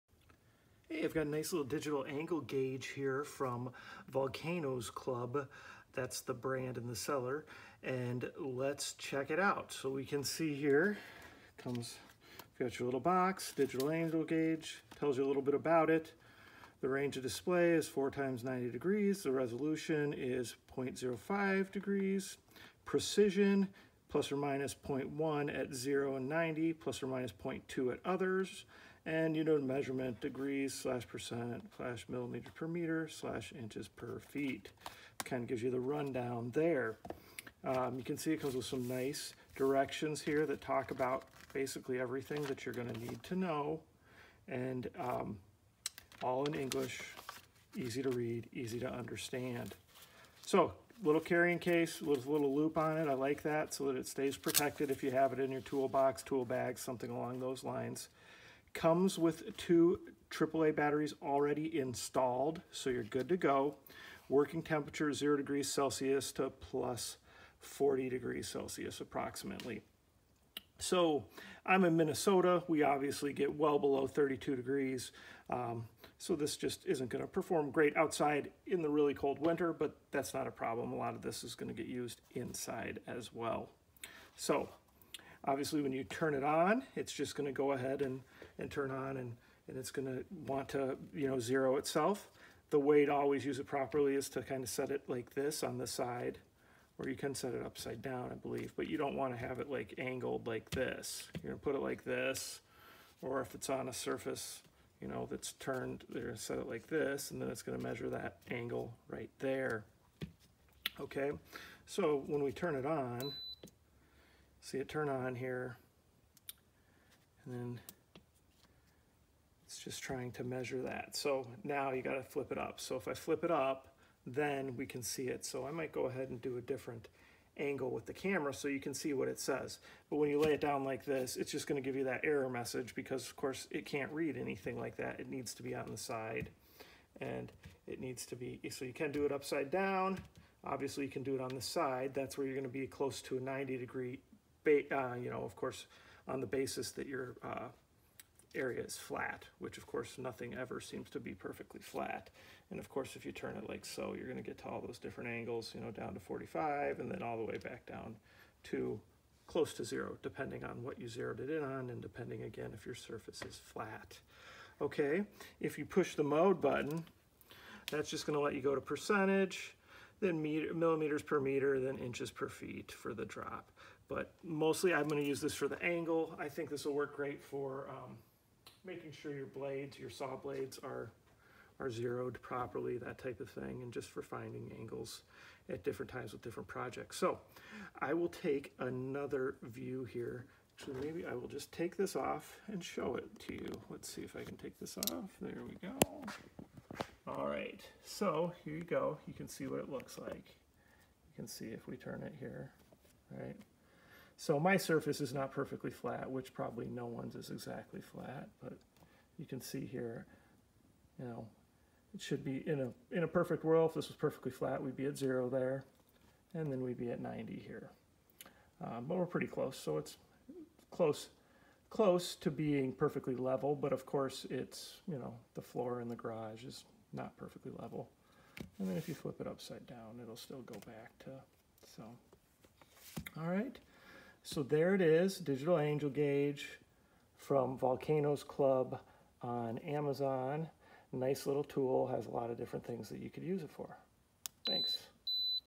Unboxing & Demo: Volcanoes Club Brand Digital Angle Finder - Angle Gauge Level
Digital Angle Finder - Angle Gauge Level Box - Level Protractor Inclinometer - Table Saw Angle Gauge Measurement Tool With Magnetic Base and Backlight for Woodworking, Construction, Masonry, Machinery

TWO MEASUREMENT MODES & FOUR UNITS: This equipment displays absolute angles and relative angles in degrees, %, mm/m and IN/FT. Turn on the equipment to directly enter the absolute measurement mode, and press the ZERO button to switch to the relative measurement mode.
EASY TO READ: The upgrade super-large LCD display with backlight and reversible display has black background and the measured value with green light, which can be clearly read in the environment with sufficient light or dark light; The measured value can be automatically turned over and displayed along with the turning over of the equipment, which is easy to read.
CALIBRATION FUNCTION: The equipment has higher resolution of 0.05°and higher accuracy of ±0.1°at 0°and 90 °, and ±0.2°at other angles. The measuring range is 4×90°; The equipment has been calibrated in the factory. In order to ensure lasting accuracy, the equipment also has calibration function, so that it can be calibrated by yourself in case of deviation of accuracy caused by various factors. Please refer to the user manual for details of calibration method.
POWERFUL BUILT-IN MAGNETIC BASE and V-GROOVE DESIGN: The powerful built-in magnetic base can be easily and firmly connected to any metal surface, including the most common table saw, which is easy to save time and effort to obtain more accurate measurement results; V-groove design can be easily placed on curved surfaces such as pipes.
MORE FEATURES: Battery power shortage prompt; Audible buzzer (the alarm will be given within the level of ±1°, and the frequency will change with the distance from the horizontal or vertical position); Automatic turn off after 3 minutes without any operation or movement to extend the battery life; The protection bag with a belt loop is attached to protect the equipment surface from wear and you can carry it anytime and anywhere.